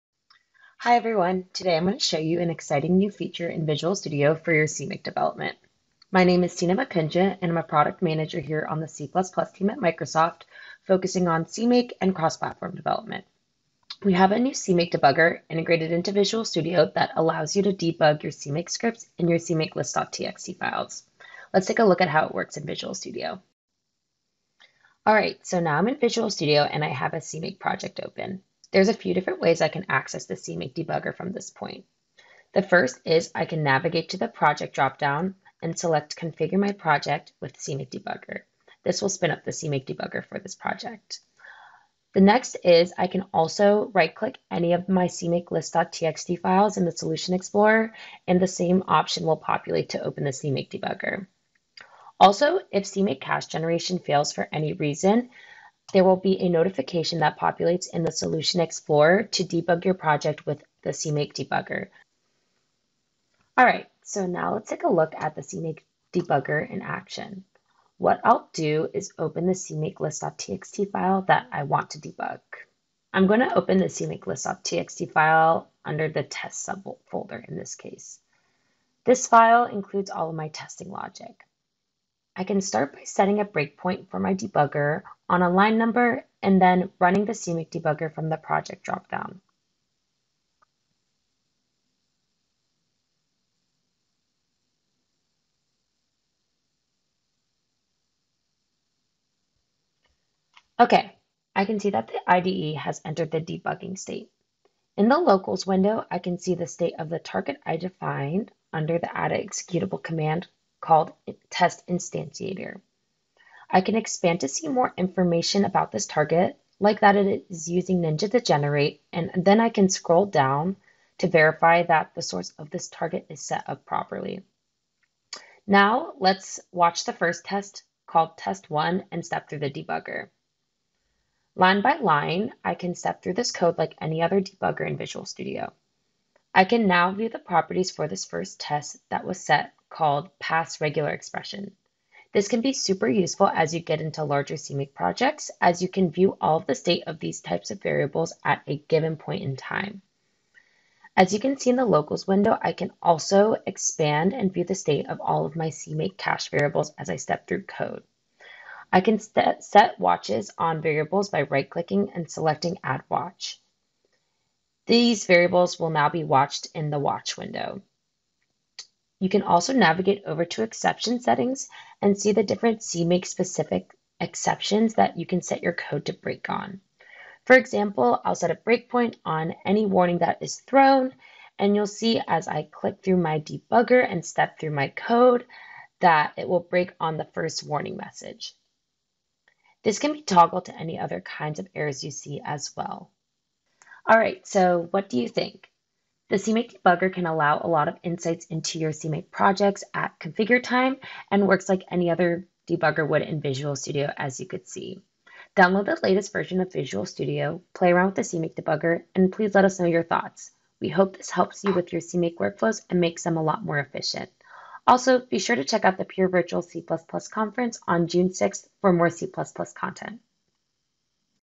CMake Debugger
Visual Studio 2022 version 17.6 comes with a new CMake Debugger for troubleshooting issues at configure-time. It acts in a very similar way to our regular code debugger and provides a familiar interface, allowing you to set breakpoints, step through the code, and visualize program state.

Find out more about the CMake Debugger in our blog post

Join us for a free, three-hour virtual conference for the whole C++ community